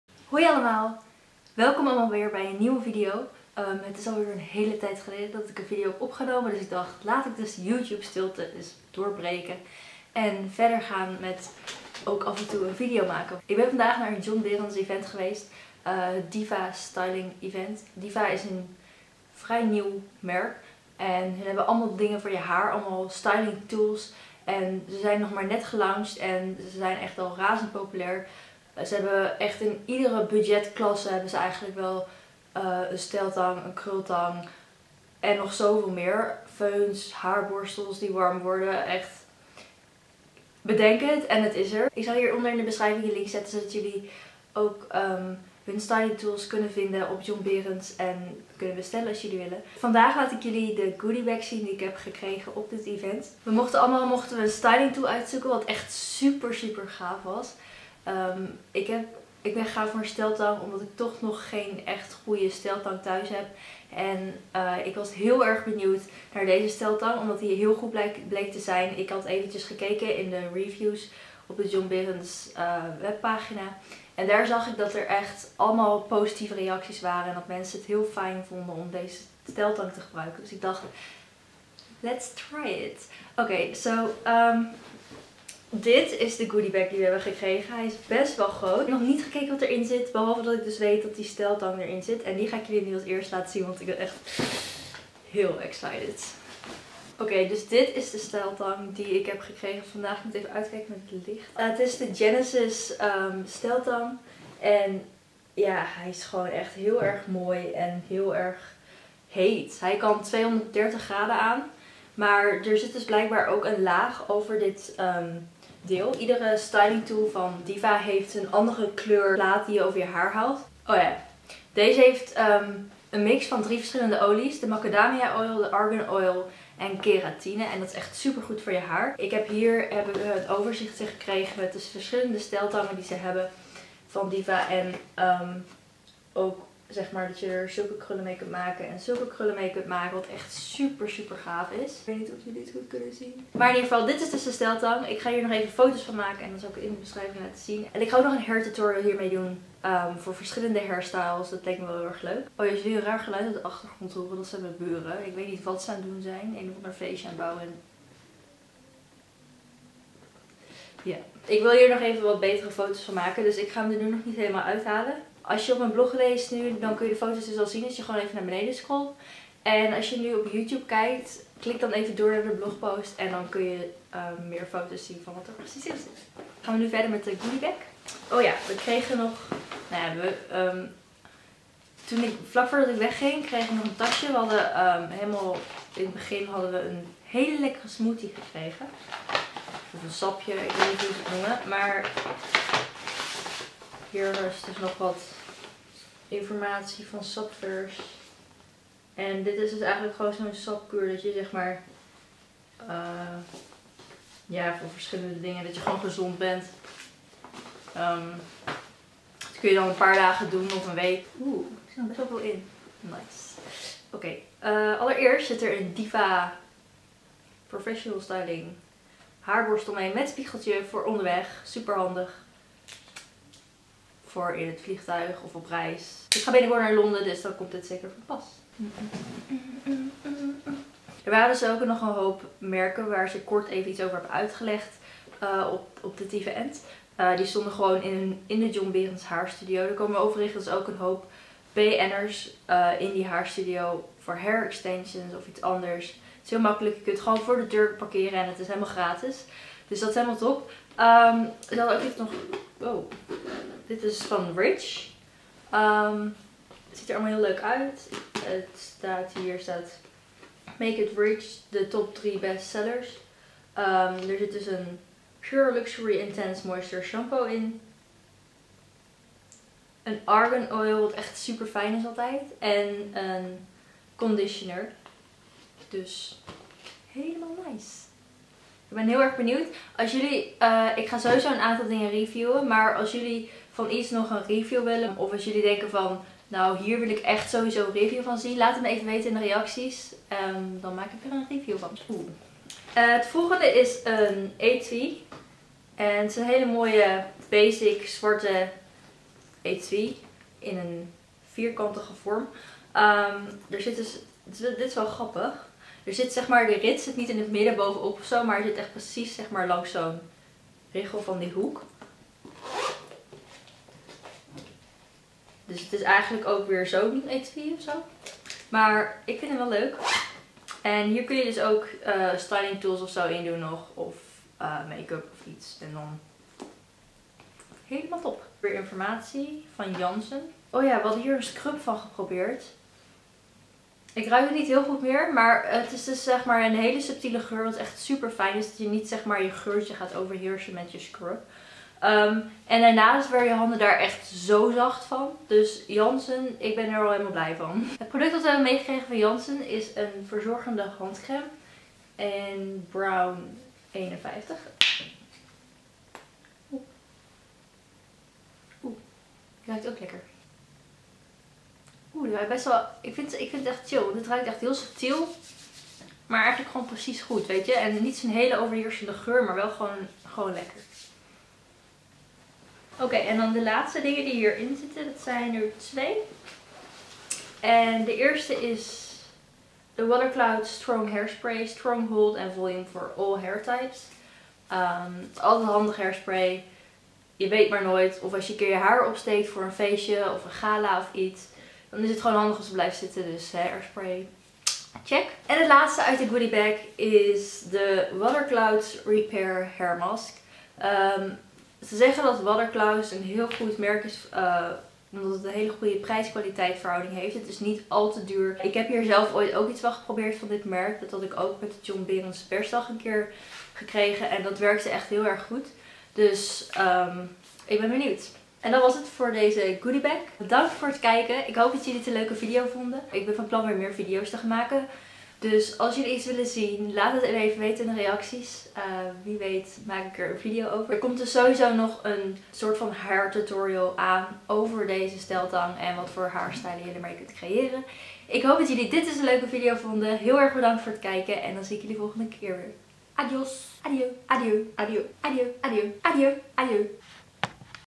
John Beerens x Diva Goodiebag Unboxing
Hey! In deze video unbox ik de goodiebag van het John Beerens x Diva Professional styling event in de John Beerens haarstudio in Tilburg! Enjoy! Xx Anna

blogfashionanna

blogfashionanna

XOXOOX